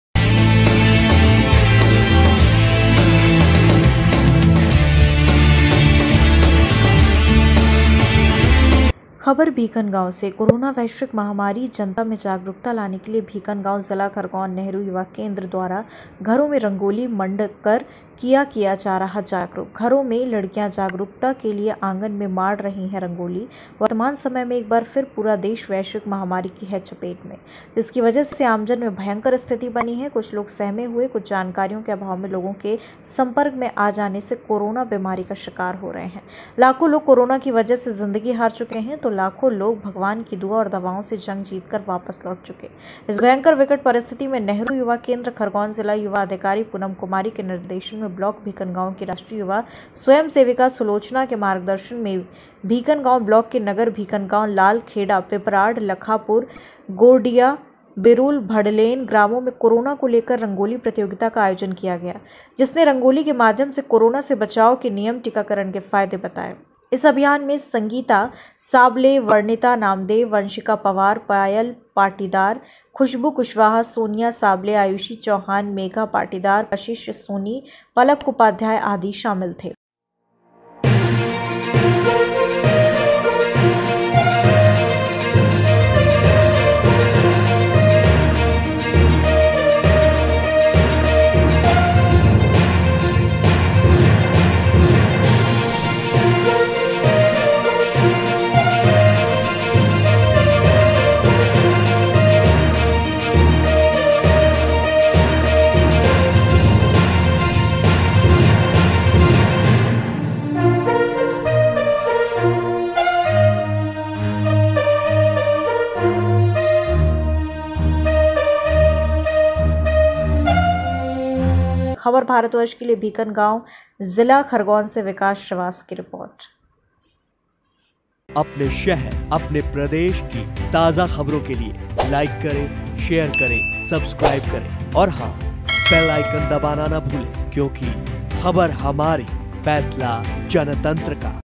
भीकनगांव : घरों में लड़कियां जागरूकता के लिए आंगन में माड रही है रंगोली
रंगोली के माध्यम से कोरोना से बचाव के नियम,टीकाकरण के फायदे बताये
 भीकनगांव, लालखेडा,पिपराड आदि ग्रामों में कोरोना को लेकर रंगोली प्रतियोगिता का आयोजन
            भीकनगांव नेहरू युवा केंद्र द्वारा घरों में रंगोली मांडकर किया जा रहा है जागरूक